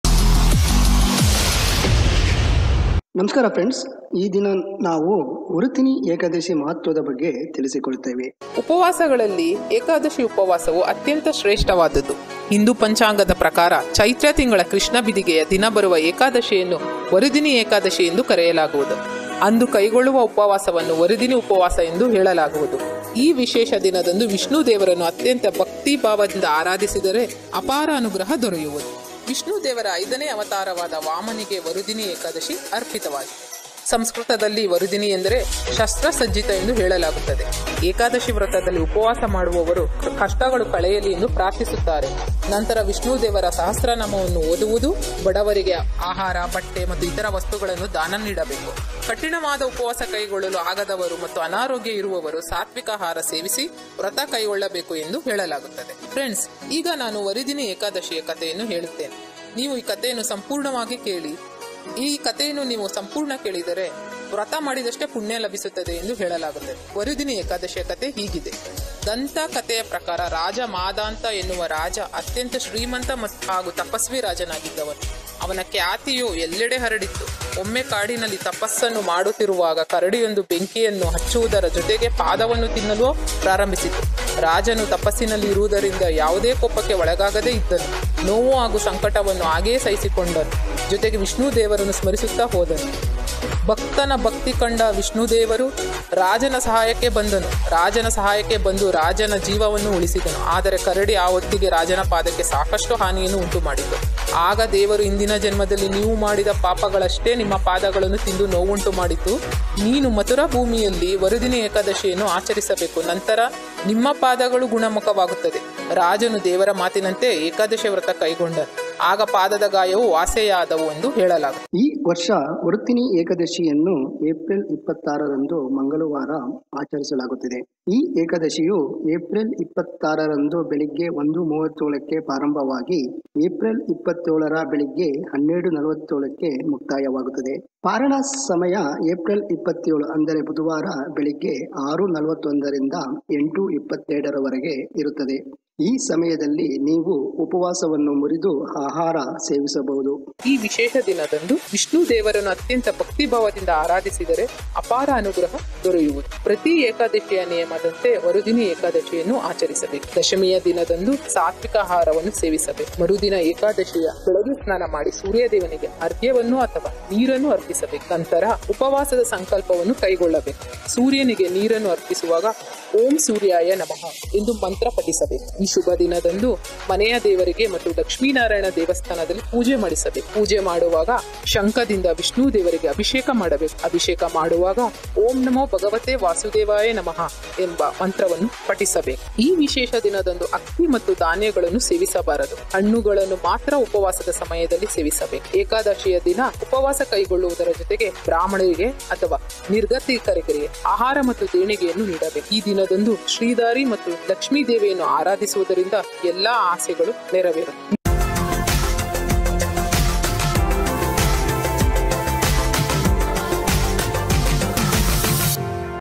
ವರುಥಿನಿ ಏಕಾದಶಿ 2022 (ಏಪ್ರಿಲ್ 26, ಮಂಗಳವಾರ)| Varuthini Ekadashi | ವರುಥಿನಿ ಏಕಾದಶಿಯ ಮಹತ್ವ | Kannada Vani
ವರುಥಿನಿ ಏಕಾದಶಿಯ ಮಹತ್ವ

ವರುಧಿನಿ ಏಕಾದಶಿಯು ಇದೇ ಏಪ್ರಿಲ್ 26,  ಶುಭ  ಮಂಗಳವಾರ ಬರುವುದು. ಏಕಾದಶಿಯ ತಿಥಿ ಏಪ್ರಿಲ್ 26  ರಂದು ಬೆಳಗ್ಗೆ  1:37ಕ್ಕೆ ಪ್ರಾರಂಭವಾಗುತ್ತದೆ. ಏಪ್ರಿಲ್ 27ರ ಬೆಳಗ್ಗೆ  12:47ಕ್ಕೆ ತಿಥಿಯು ಮುಕ್ತಾಯವಾಗುವುದು.
ಪರಾನ ಸಮಯ :   ಏಪ್ರಿಲ್ 27ರ ಮುಂಜಾನೆ 6:41 ಗಂಟೆಯಿಂದ ಬೆಳಿಗ್ಗೆ 8:22 ರವರಿಗೆ ಇರುತ್ತದೆ

- Kannada Vani